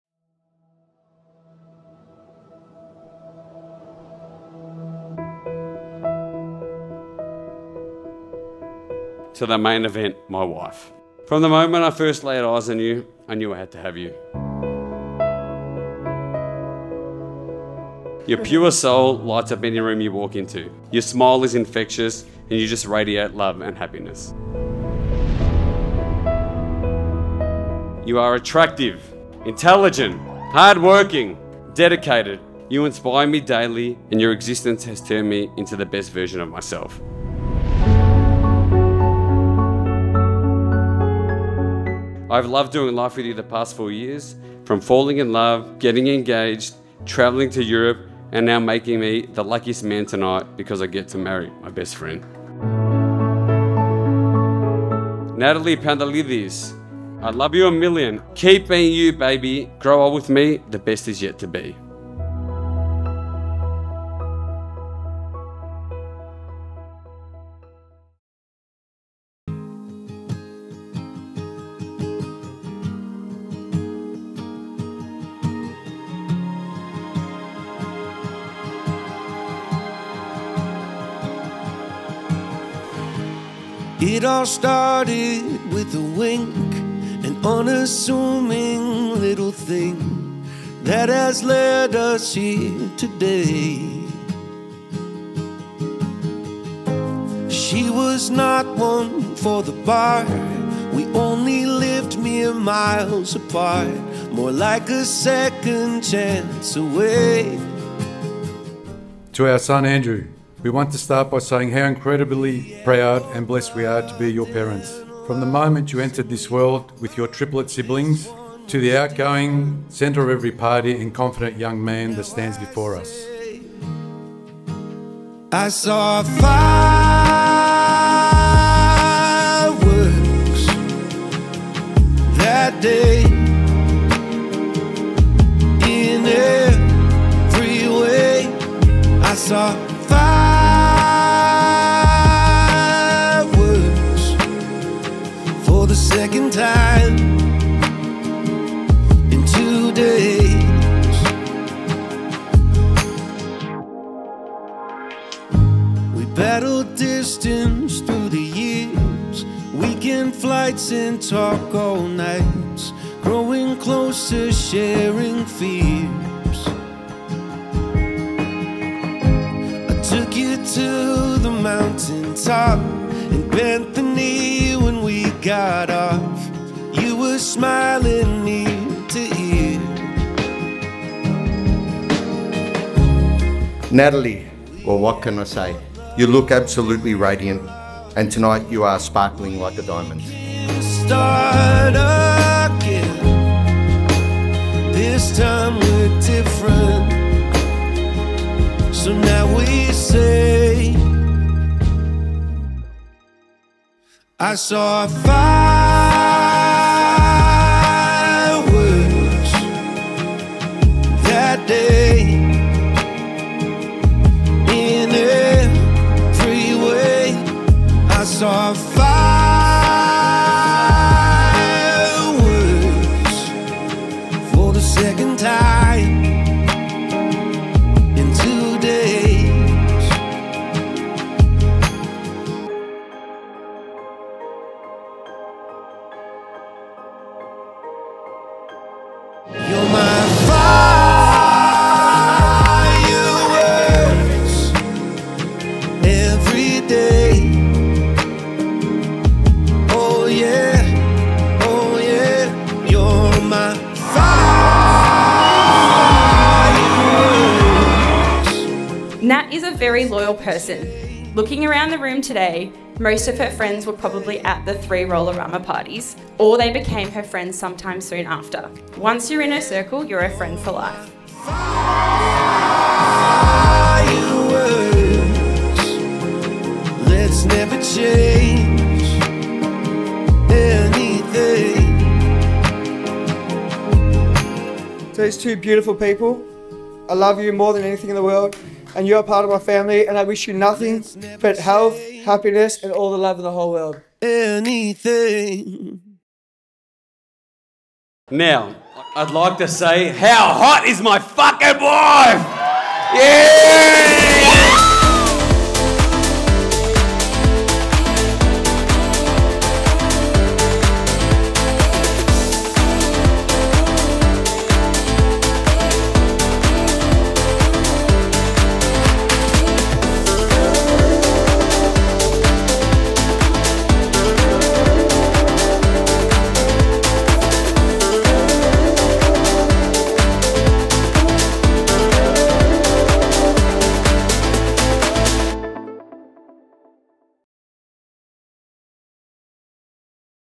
Natalie and Andrew - Carousel
The beautiful wedding of Natalie and Andrew - Carousel in Melbourne, Australia.

Filmed and edited by Momento Films.